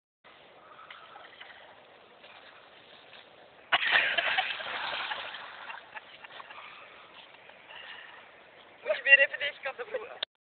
Hugo zwemt
Our dog Hugo jumps in to have a swim...